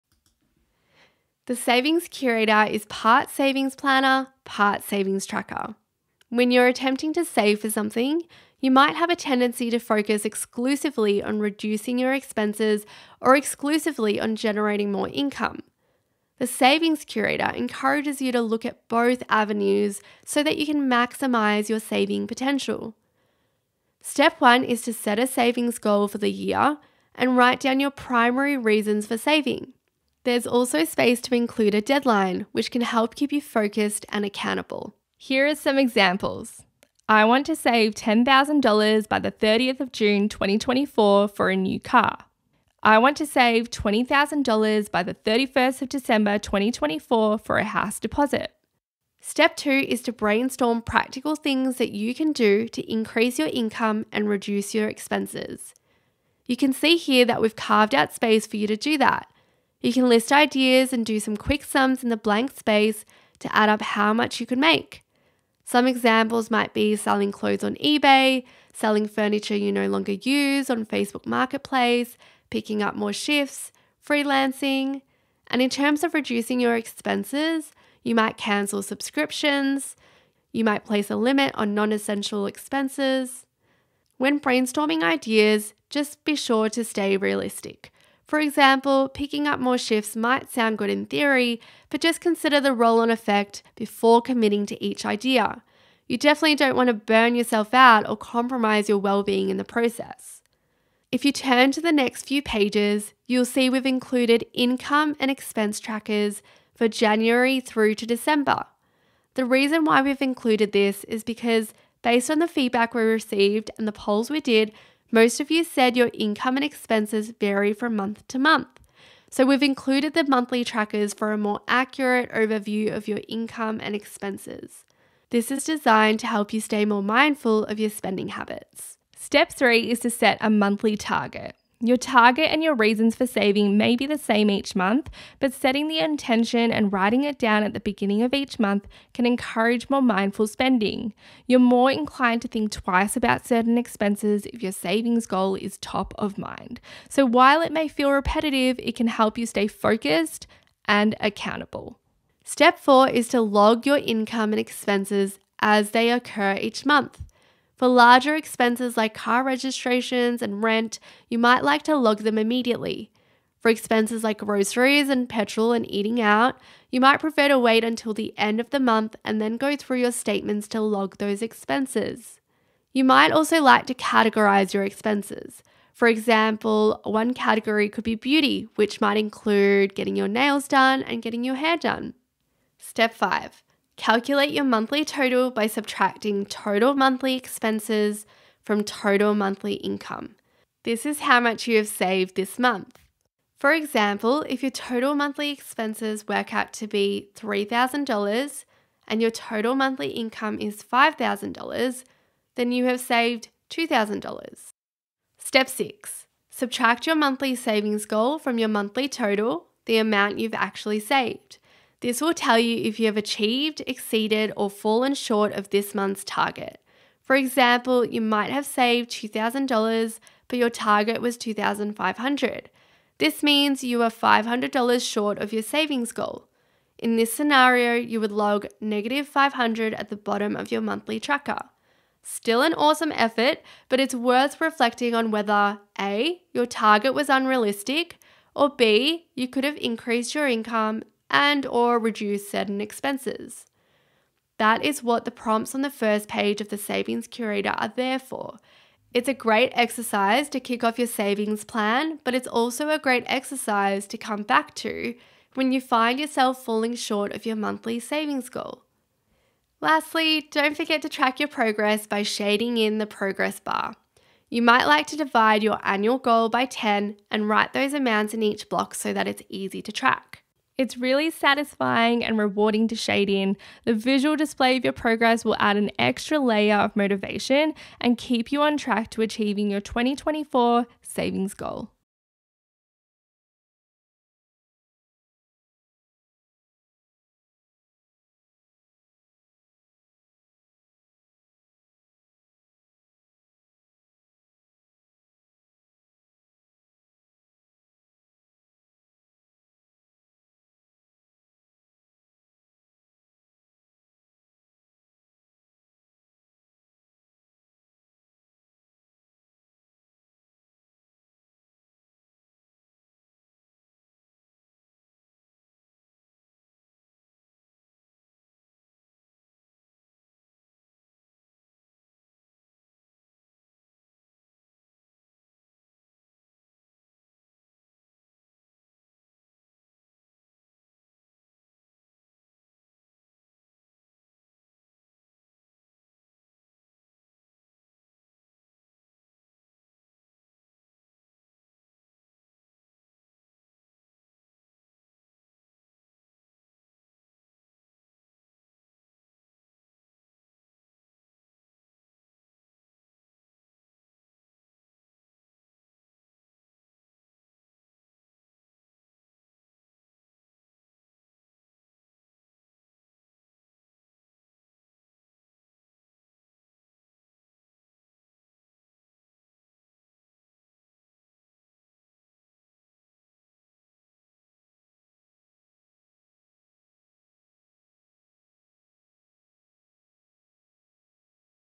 Curation 2024 Planner How To - Savings Curator
Learn how to use the Savings Curator in Curation 2024 Planner.